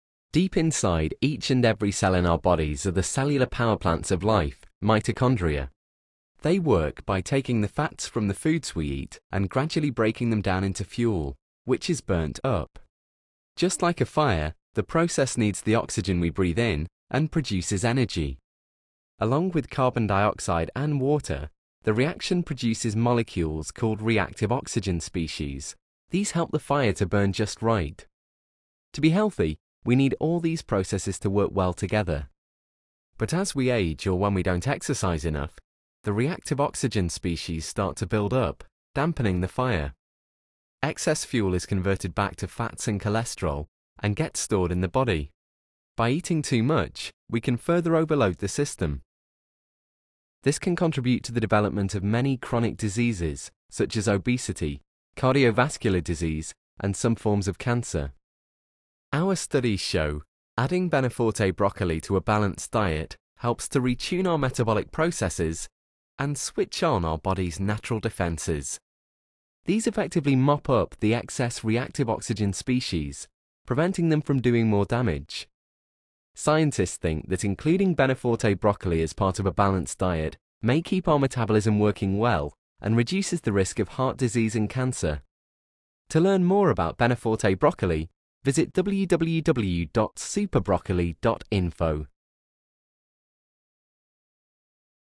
Super Broccoli
Each cell in our body contains the central metabolic machinery which converts the carbohydrates and fats in our food to energy and other useful compounds needed to function healthily. As we age or when our cells become stressed, our metabolic machinery starts to work less well and begins to make what are known as reactive oxygen species (ROS). While a healthy cell needs a small amount of ROS, too much can be damaging. By having regular doses of glucoraphanin, we can keep our ROS within safe limits, which helps our metabolism to function normally. One consequence of keeping ROS in balance is that we can burn up fat effectively, rather than storing it in our bodies.